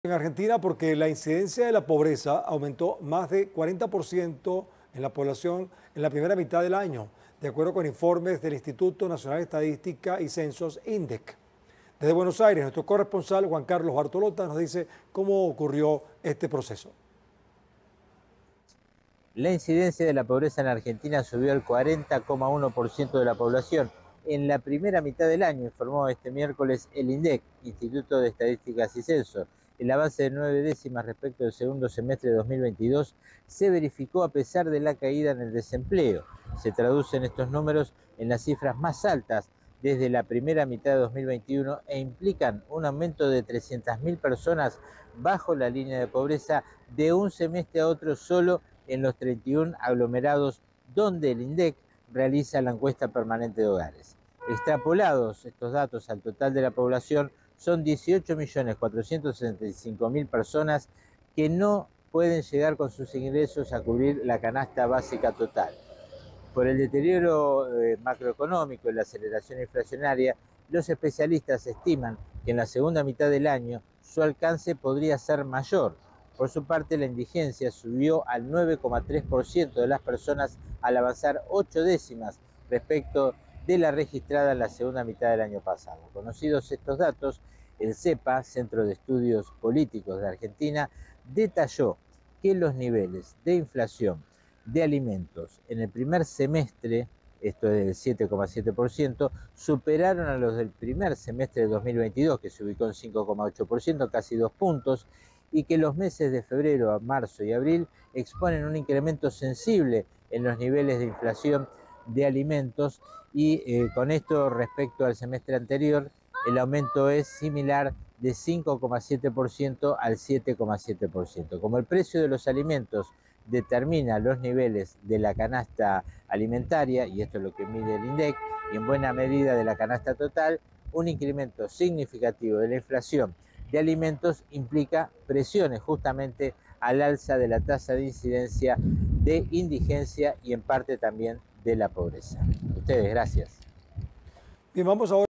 Argentina: Informes del INDEC revelaron que la pobreza aumentó un 41.01%
Se registran las cifras más altas desde la primera mitad del 2021, lo cual implica un aumento de 300 mil personas bajo la línea de pobreza. teleSUR